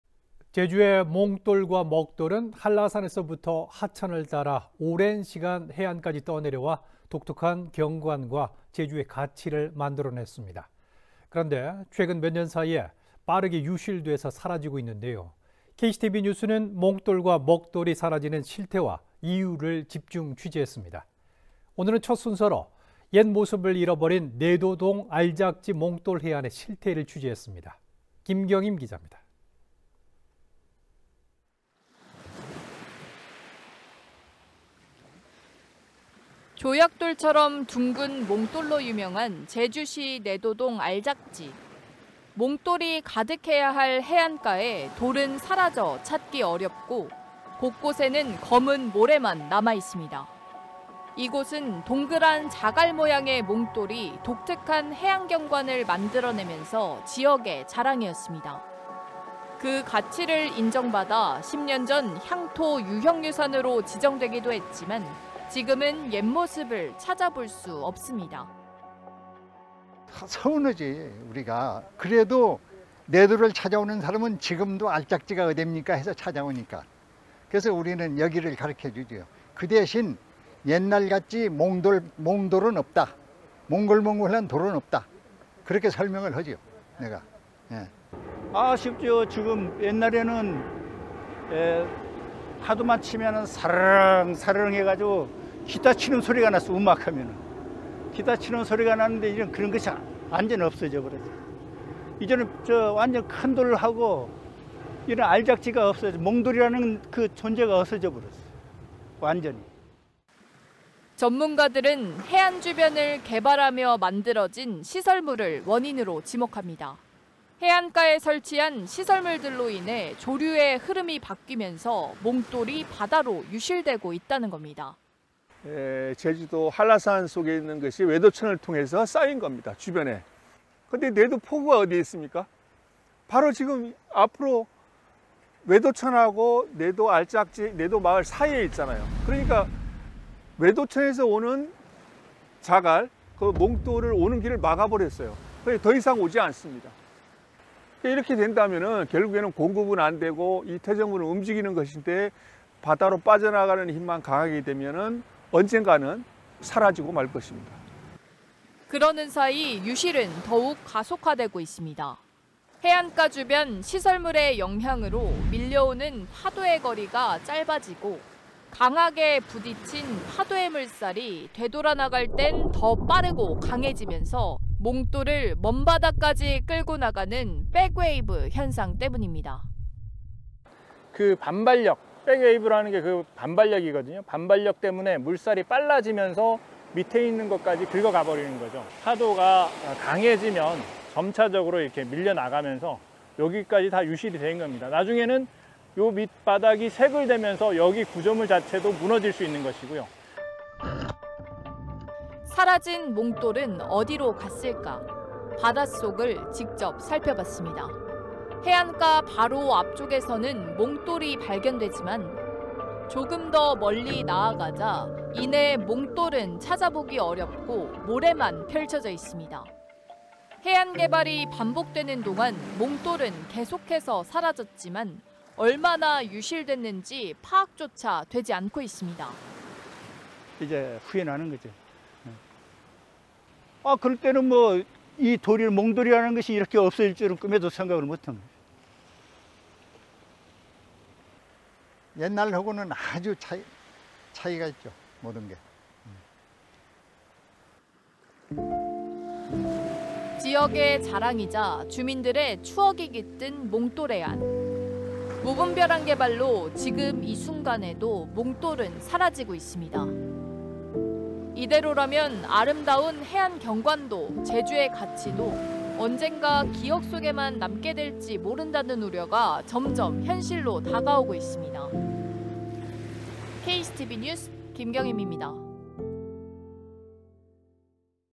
돌 기획 ① 무분별한 개발에 사라지는 몽돌
제주의 몽돌과 먹돌은 한라산에서부터 오랜시간 내려와 독특한 해안 경관을 만들어내며 제주의 가치와 역사를 지니고 있습니다.

그런데 최근 몇년 사이 몽돌과 먹돌이 빠르게 사라지고 있습니다.

KCTV제주방송은 제주의 가치를 지닌 몽돌과 먹돌이 사라지는 실태와 이유를 집중 취재했습니다.

첫 순서로는 빠르게 유실되고 있는 내도동 알작지 몽돌해안의 실태를 전해드립니다.